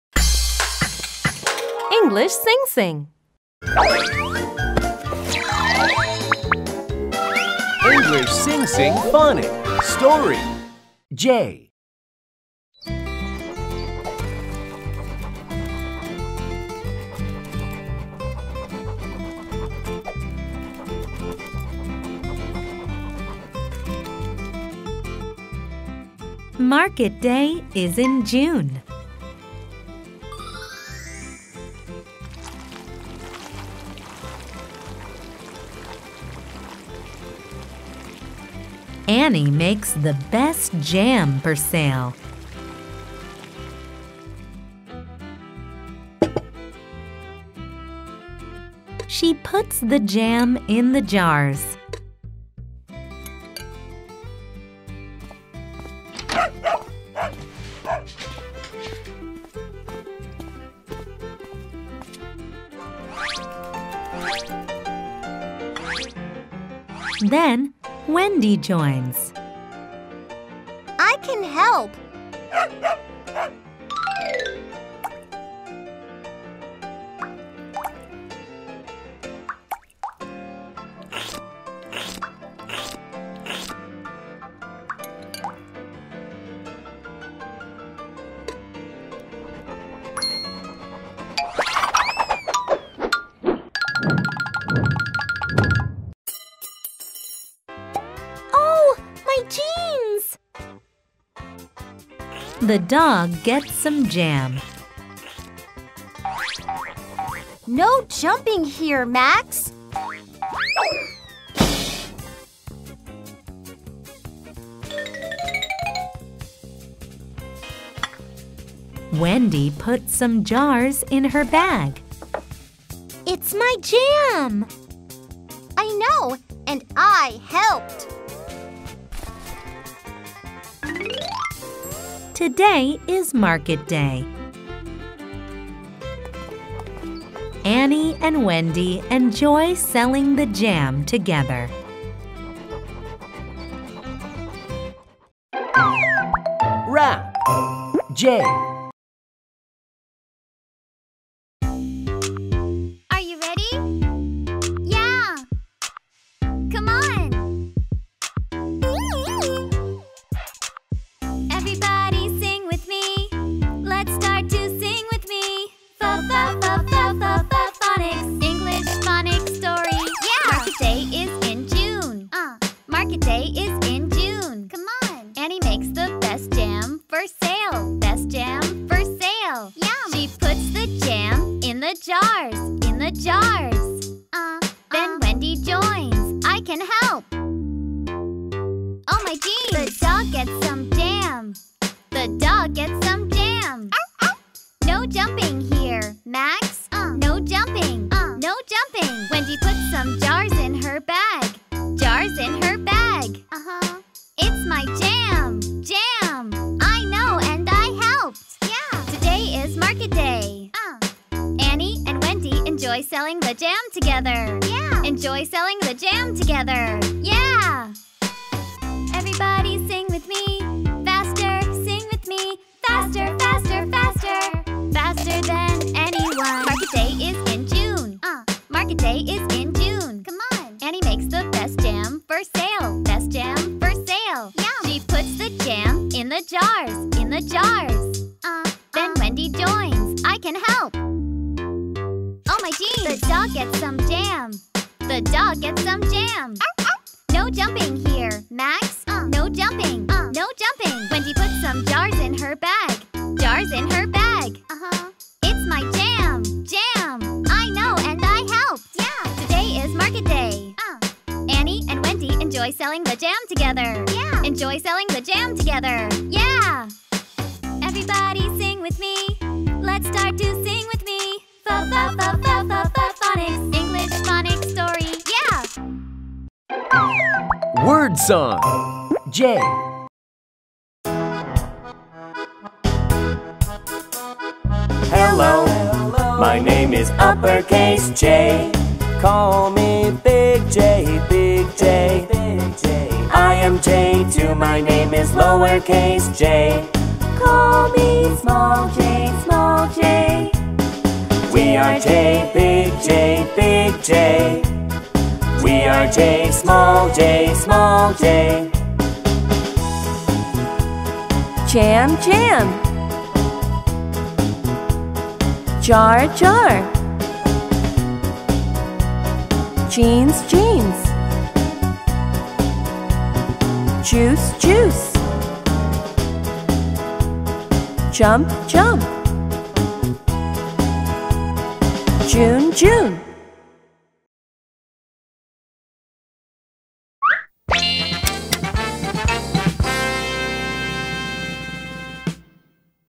Phonics 'J' Collection - Alphabet Bundle - Educational video for Kids
Learn more about the letter “J” by listening to stories and singing the songs! Let’s learn and have fun at the same time!

-- Title: Phonics 'J' Collection --